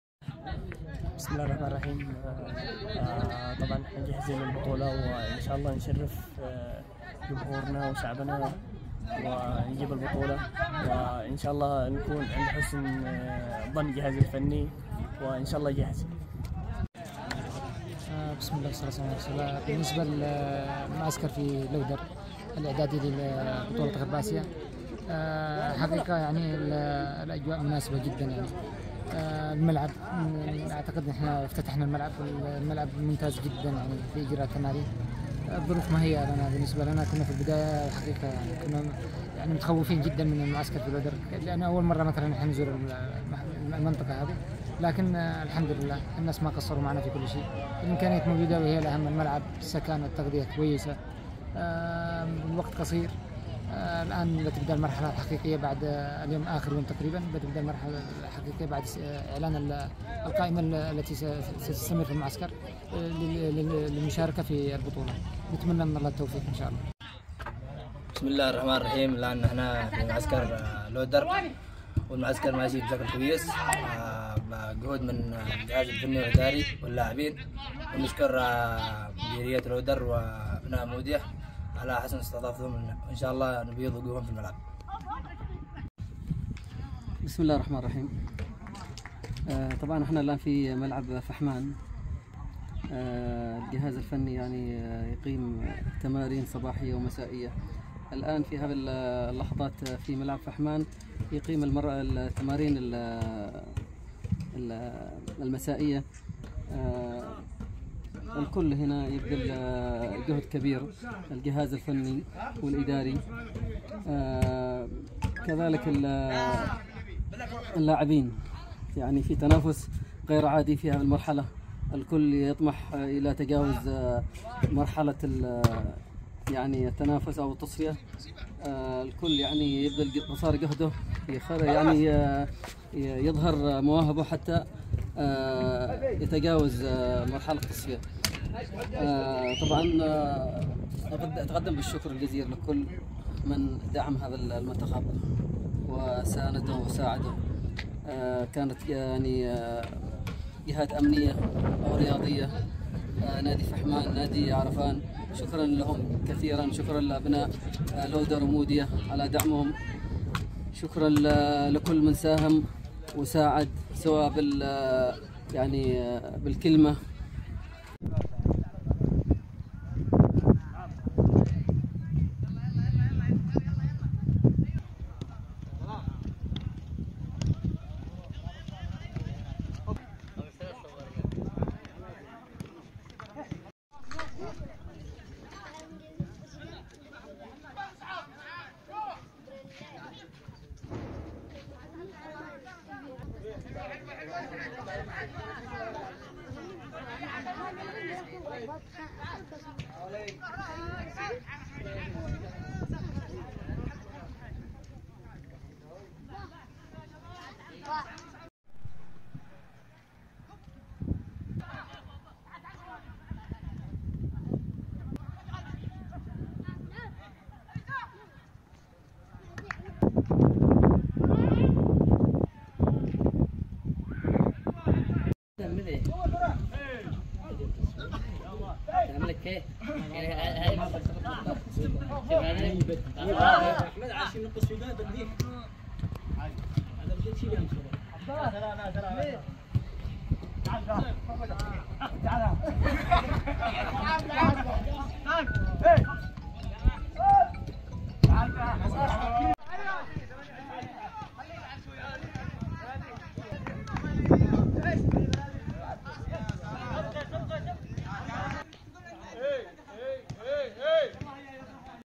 لاعبو المنتخب اليمني للشباب في معسكرهم الإعدادي بابين قبل إعلان القائمة 28 لاعب
لقطات من تدريبات المنتخب الوطني للشباب في معسكره الاعدادي الداخلي بمحافظة أبين إستعداداً لبطولة غرب آسيا بالسعودية

تصوير أحمد الحامدي